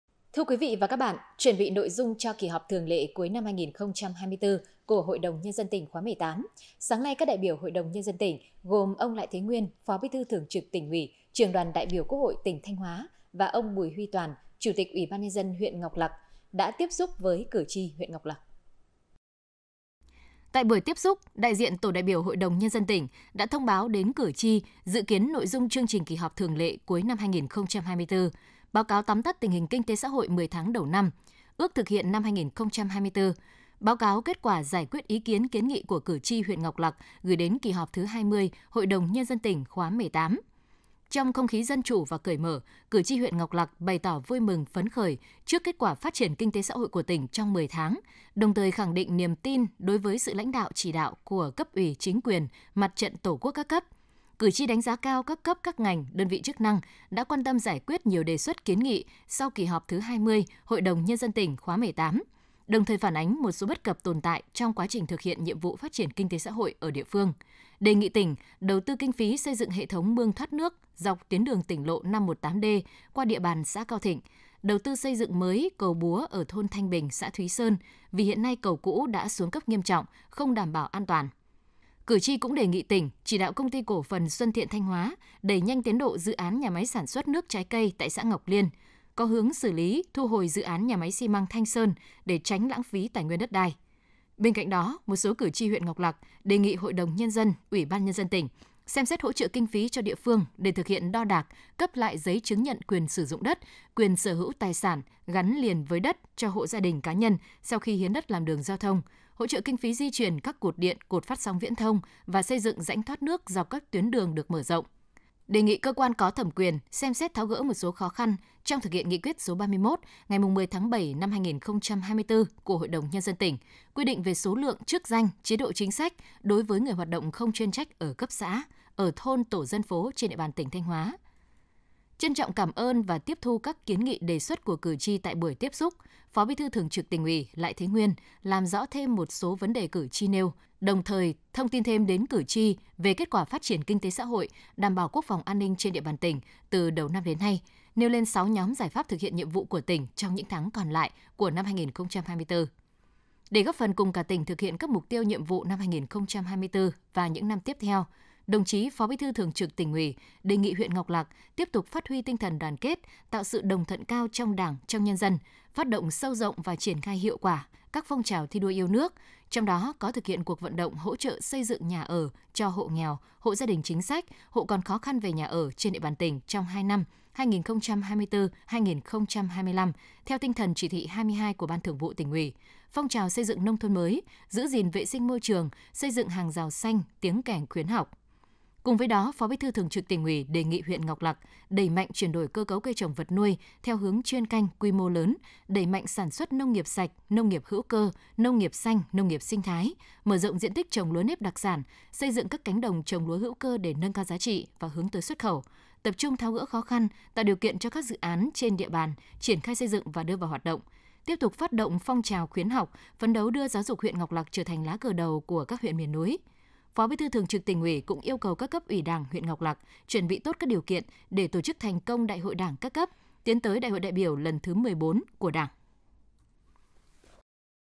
Phó Bí thư Thường trực Tỉnh uỷ Lại Thế Nguyên tiếp xúc cử tri tại huyện Ngọc Lặc
Chuẩn bị nội dung cho kỳ họp thường lệ cuối năm 2024 của HĐND tỉnh khoá XVIII, sáng ngày 2/11, các Đại biểu HĐND tỉnh gồm ông Lại Thế Nguyên, Phó Bí thư Thường trực Tỉnh uỷ, Trưởng đoàn Đại biểu Quốc hội tỉnh Thanh Hóa và ông Bùi Huy Toàn, Chủ tịch UBND huyện Ngọc Lặc, đã tiếp xúc với cử tri huyện Ngọc Lặc.